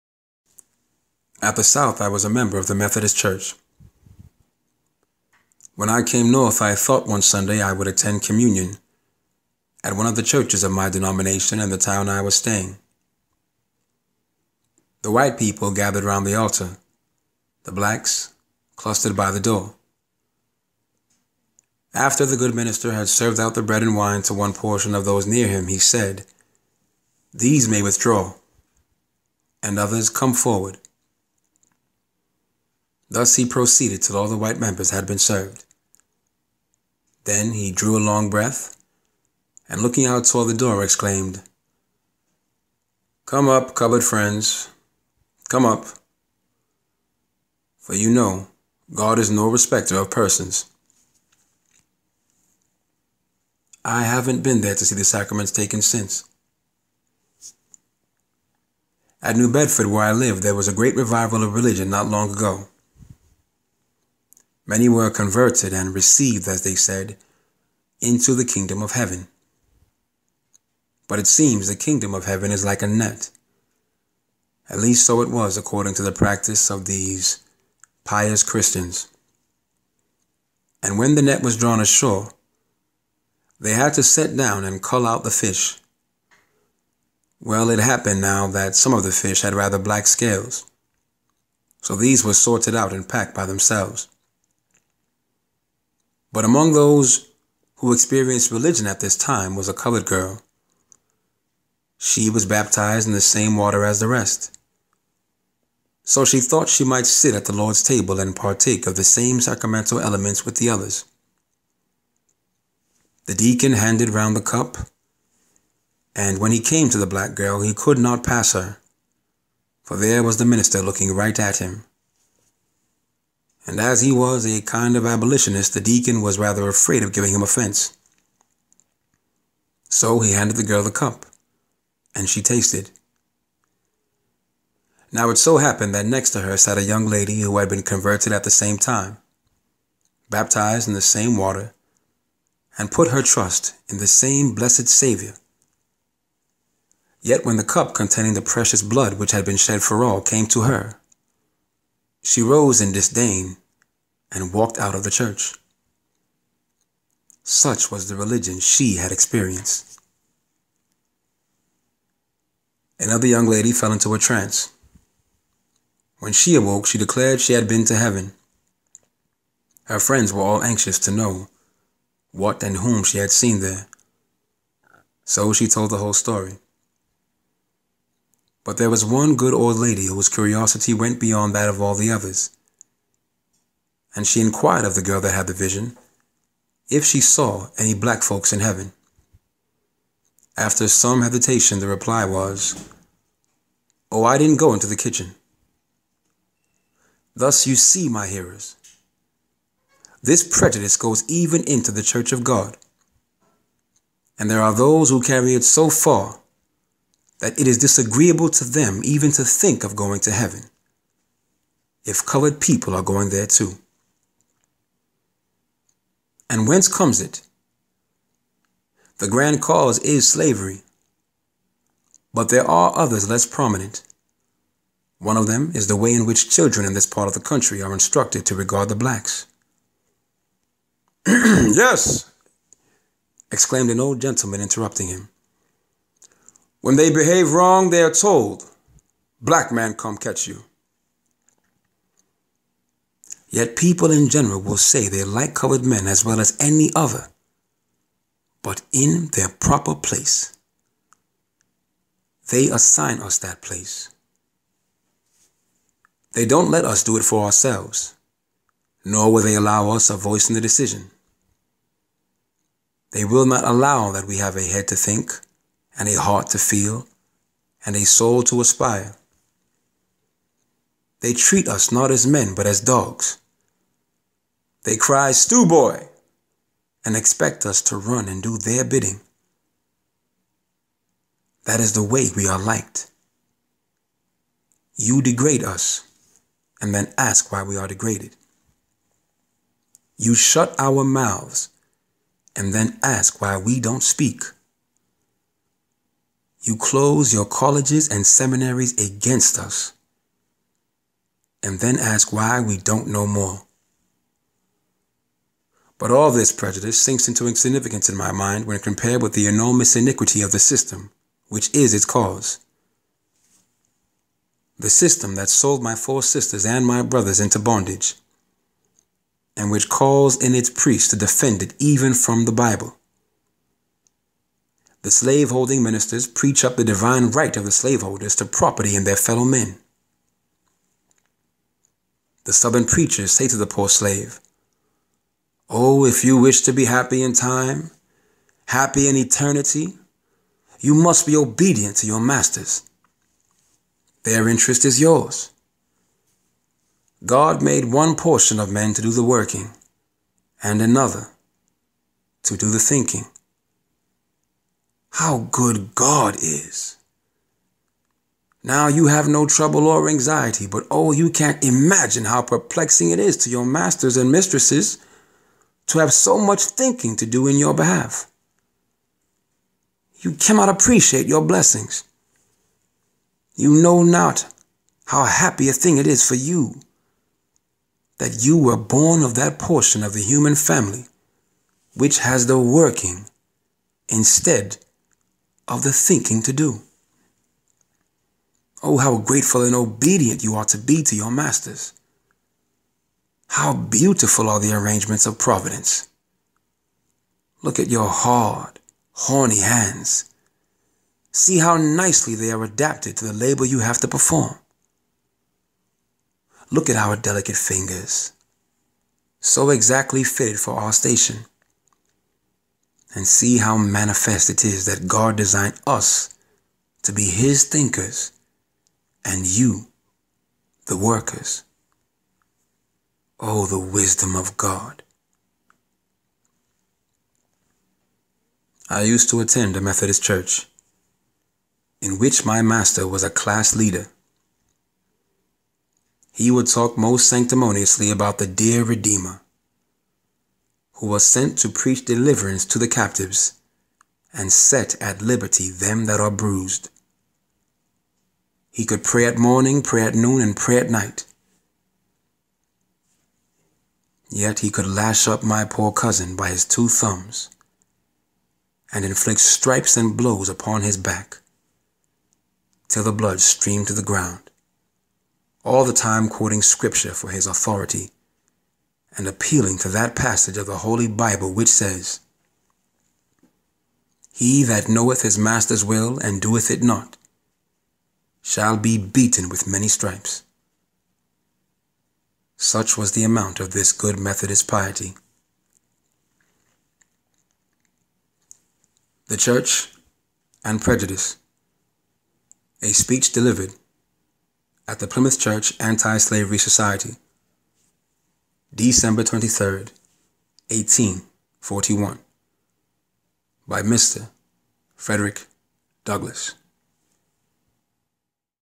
"The Church and Prejudice," by Frederick Douglass (Orated by Victor Kakulu)
"The Church and Prejudice," a speech by Frederick Douglass. A speech delivered at the Plymouth Church Anti-Slavery Society, December 23, 1841. Orated by Victor Kakulu.